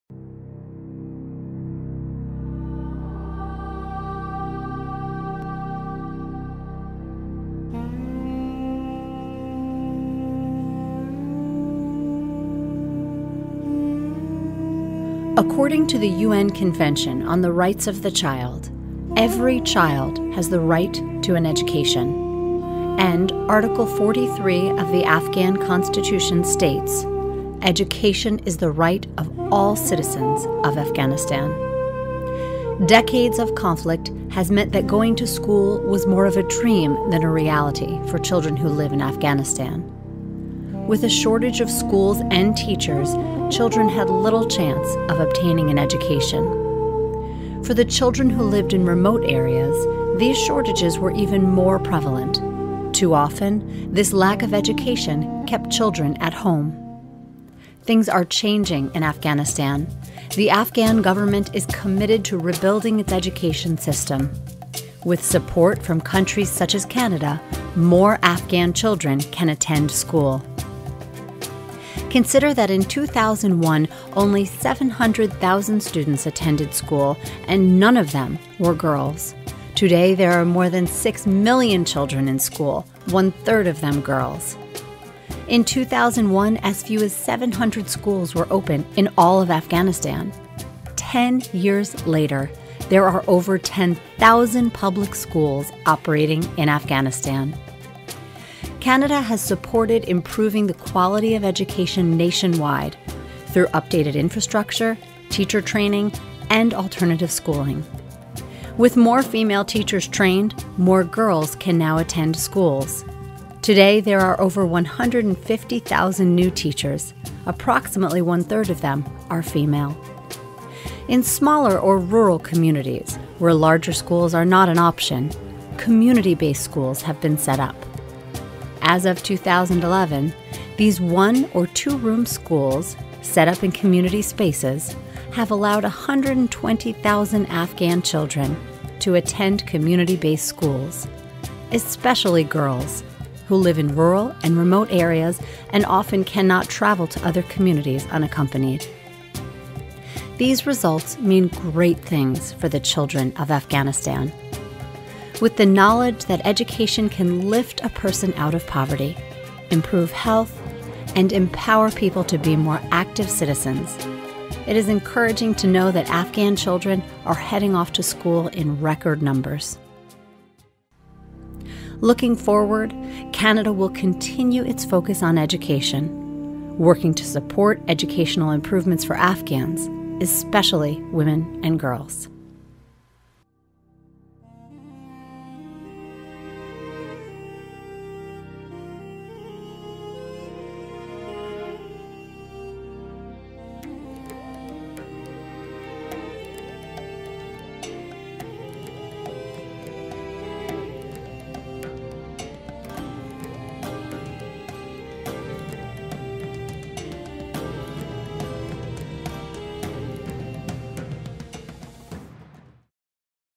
Canada Supports Education in Afghanistan
According to the UN Convention on the Rights of the Child: "Every child has the right to an education," and, Article Forty-Three of the Afghan Constitution states: "Education is the right of all citizens of Afghanistan".

Decades of conflict has meant that going to school was more of a dream than a reality for children who live in Afghanistan. With a shortage of schools and teachers, children had little chance of obtaining an education. For the children who lived in remote areas, these shortages were even more prevalent. Too often, this lack of education kept children at home.